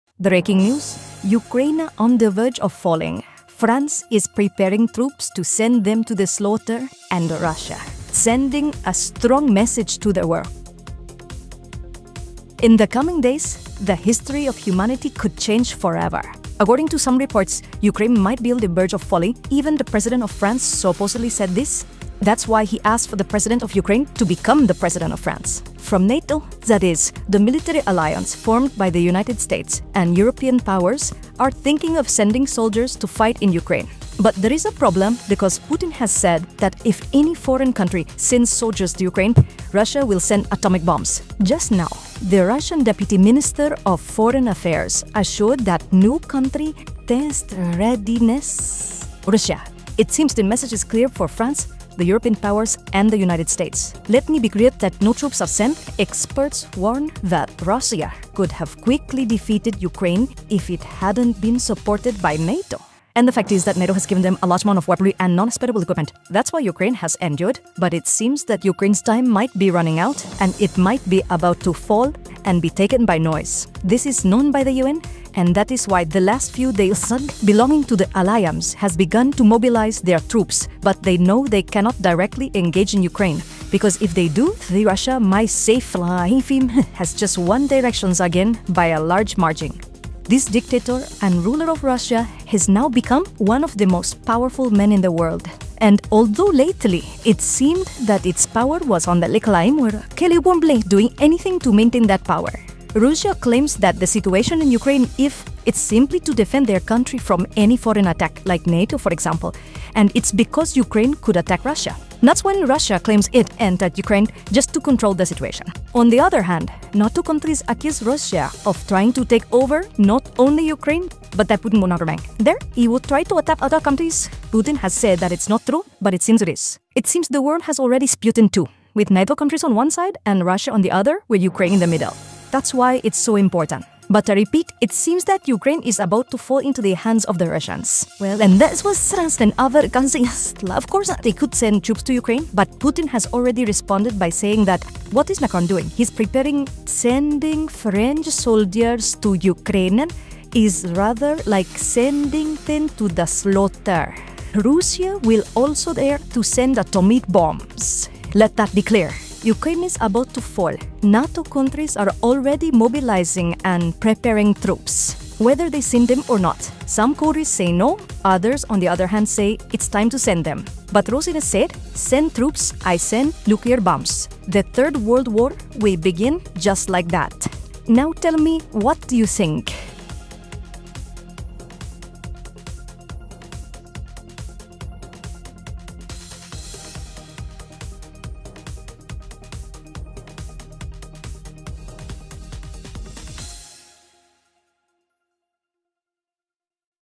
Russia Sends Strong Message as Ukraine Teeters on the Edge
Ukraine is on the verge of falling, and France is preparing to send troops while Russia sends a strong message to the world. The Deputy Minister of Foreign Affairs of Russia commented that no country should test Russia's preparedness, and it seems that this message was clear to the European Powers and the USA. Ukraine is on the brink of being taken by Russia, and the European Union knows it, but they are abstaining due to Putin's clear threat.